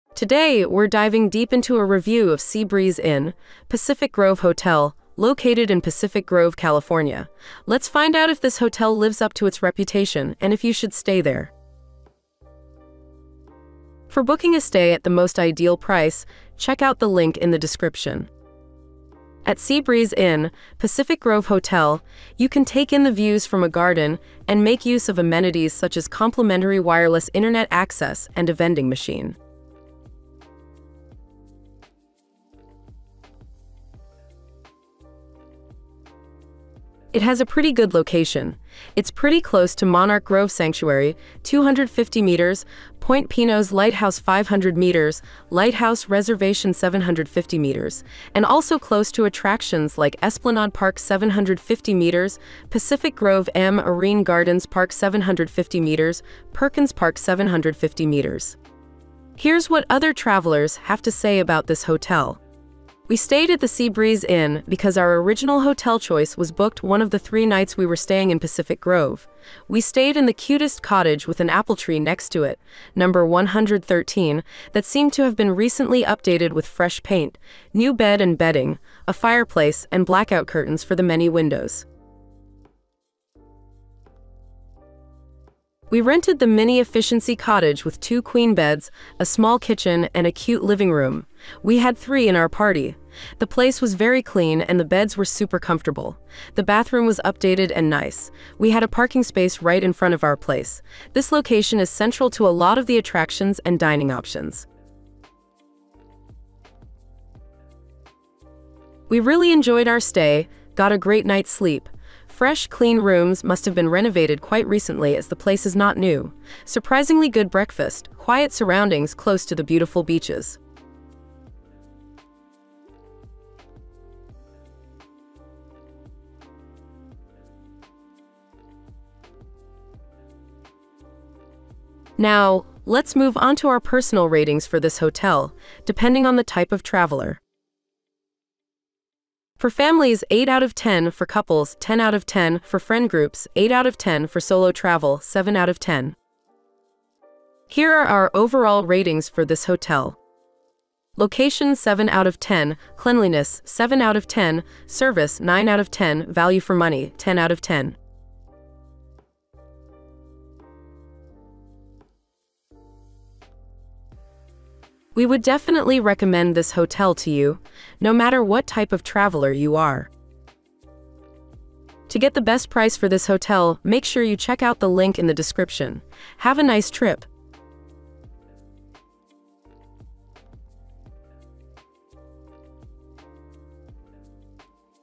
Sea Breeze Inn Pacific Grove Hotel Review - Is It Worth The Price?
In this video, we reviewed the Sea Breeze Inn - Pacific Grove based on its price, quality, value for money, and more. We also gave it ratings based on the travel type and more.

 Additionally, we'll provide you with insights into the nearby attractions and activities that you can enjoy during your stay at Sea Breeze Inn - Pacific Grove. Whether it's exploring the local culture, sightseeing, or shopping, we've got you covered.